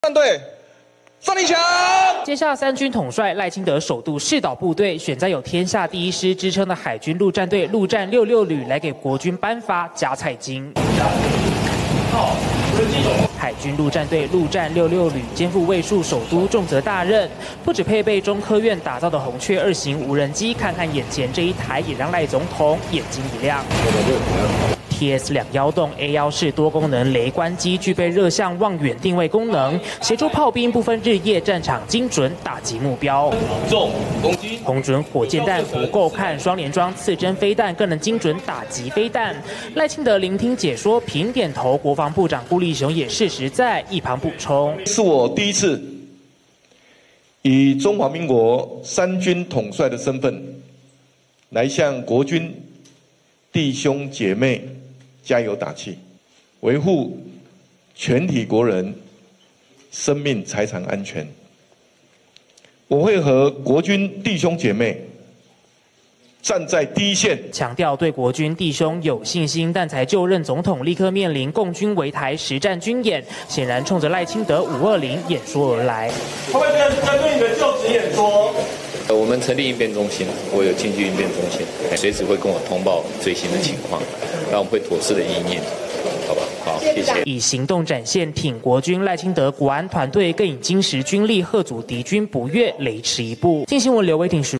共軍圍台之際 賴首以三軍統帥身分赴「天下第一師」｜鏡新聞
總統賴清德今日（5/23）下午，首度以「三軍統帥」身分，前往桃園「海軍陸戰隊 陸戰六六旅」勗勉國軍弟兄，並強調他對官兵充滿信心。此時此刻，面臨中共解放軍圍台軍演，國防部長顧立雄證實，中央已經成立應變中心，隨時掌握敵軍動向。